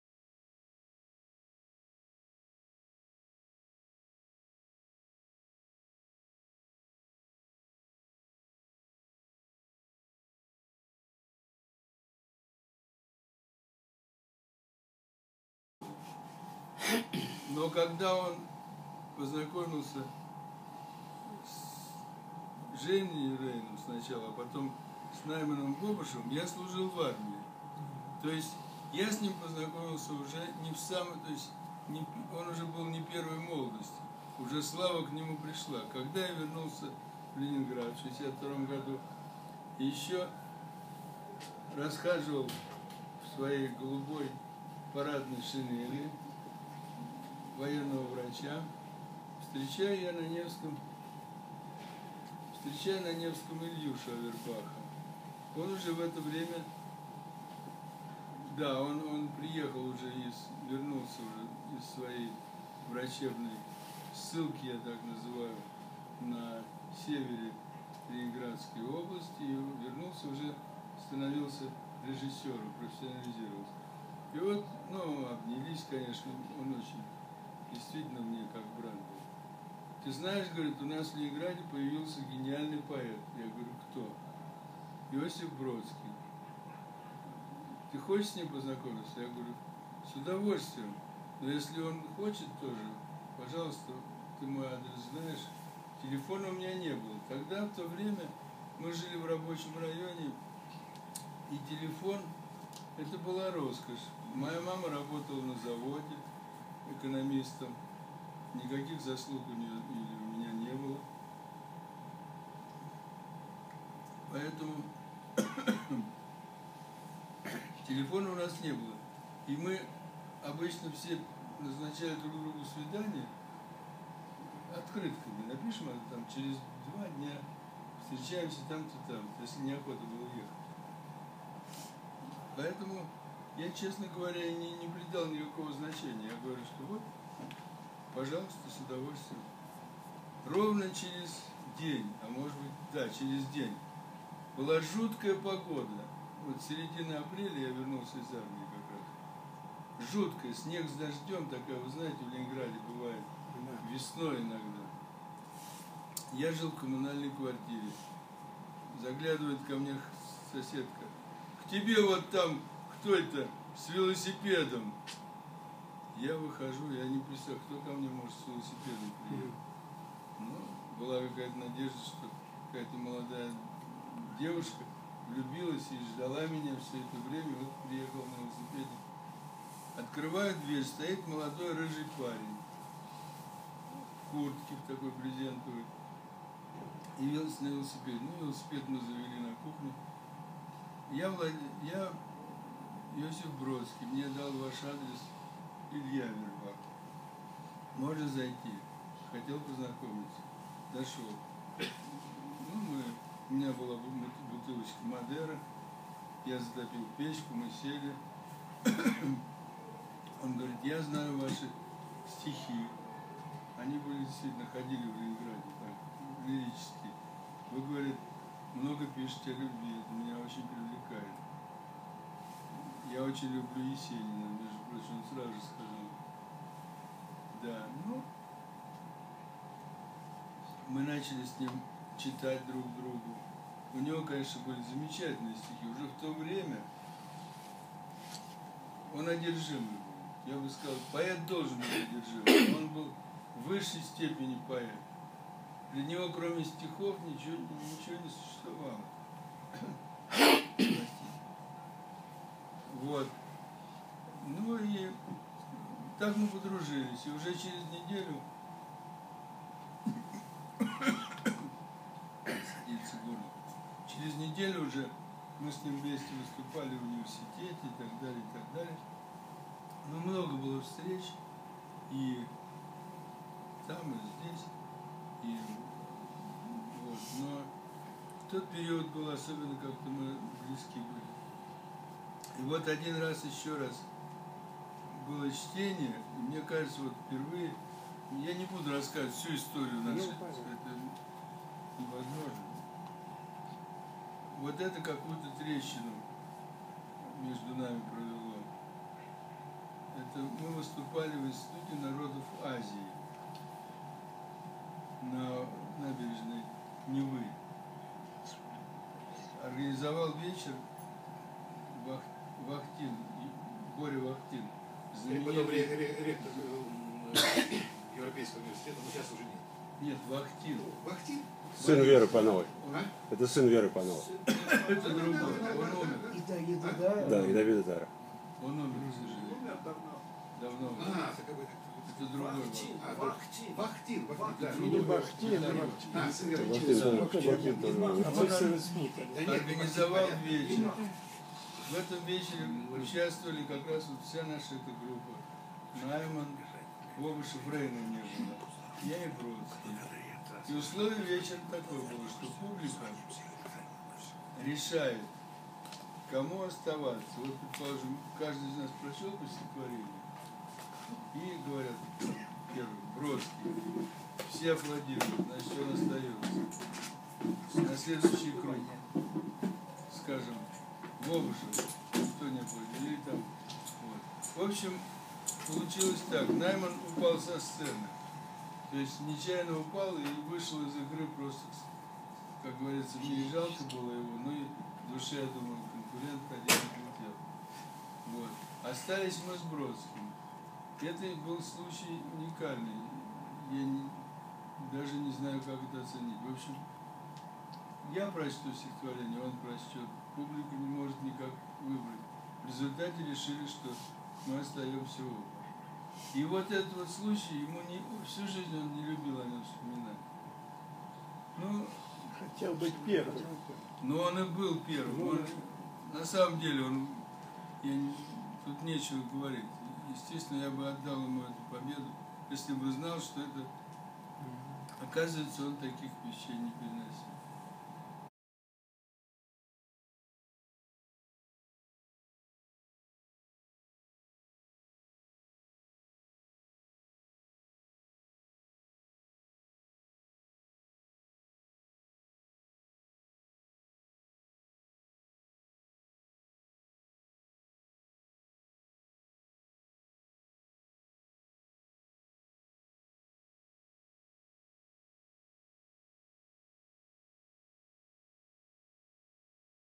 David Shrayer-Petrov. Metings with Joseph Brodsky (1960s).
David Shrayer-Petrov, Meetings with Joseph Brodsky (1960s). Recorded at the Jerusalem City Russian Library on 20 December 2012.
Давид Шраер-Петров, Встречи с Иосифом Бродским (1960-е).
Записано в Иерусалимской городской русской библиотеке
20 декабря 2012 года.